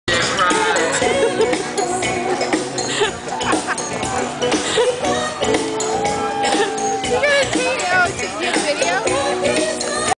Sherwood sisters dancing
the Sherwood sisters party it up on RAGBRAI